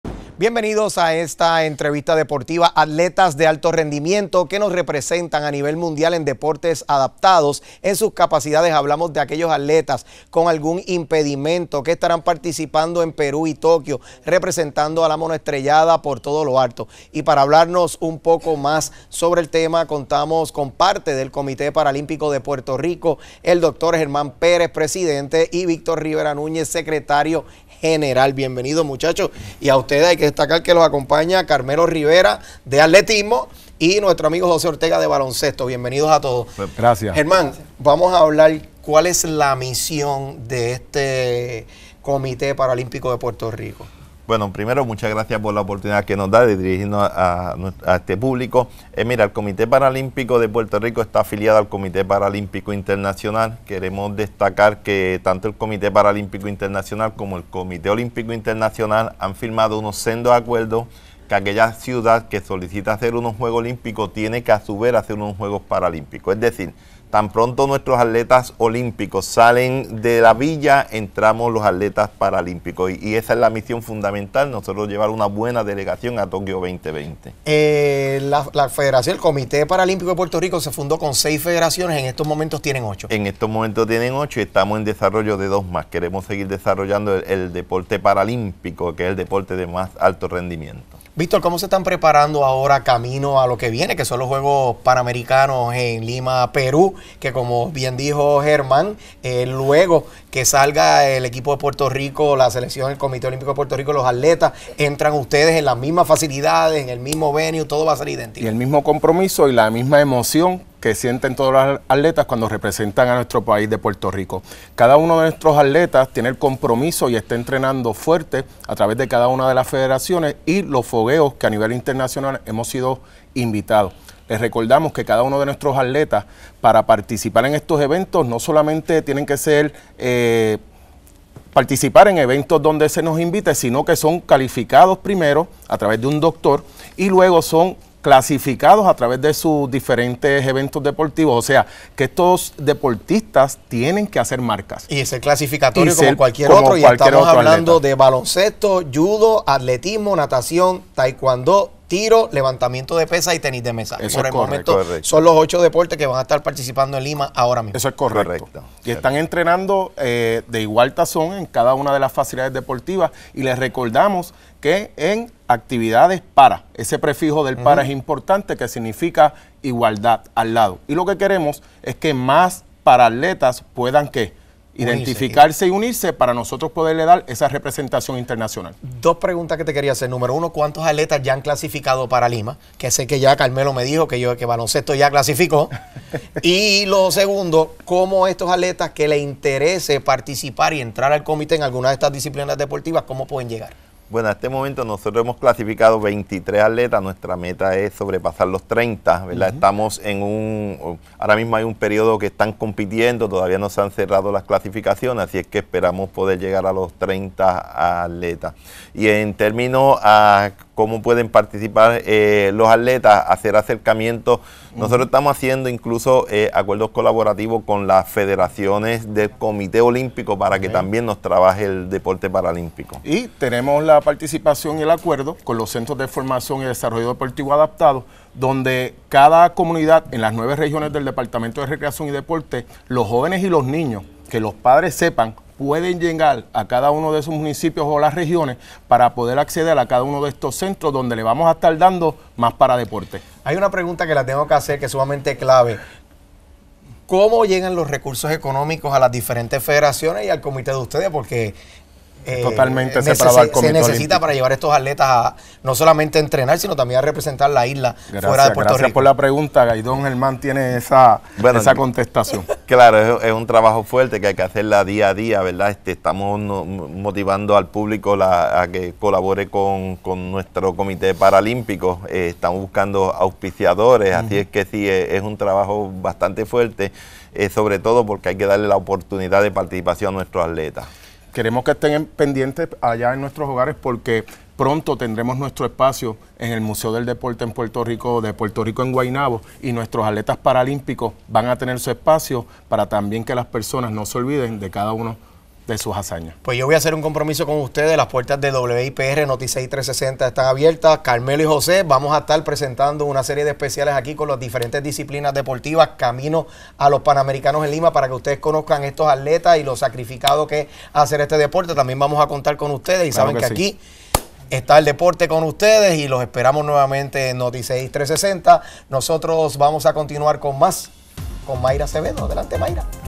Atletas boricuas listos para evento clasificatorio de Juegos Olímpicos y Paralímpicos Tokio 2020
Atletas de alto rendimiento que nos representan a nivel mundial en deportes adaptados en sus capacidades. Hablamos de aquellos atletas con algún impedimento que estarán participando en Perú y Tokio representando la mono estrellada por todo lo alto. Los miembros del Comité Paralímpico de Puerto Rico, el doctor, Germán Pérez Rodríguez, presidente, Víctor Rivera Núñez, secretario general, y Annette Jiménez, jefa de misión nos ofrecen más detalles.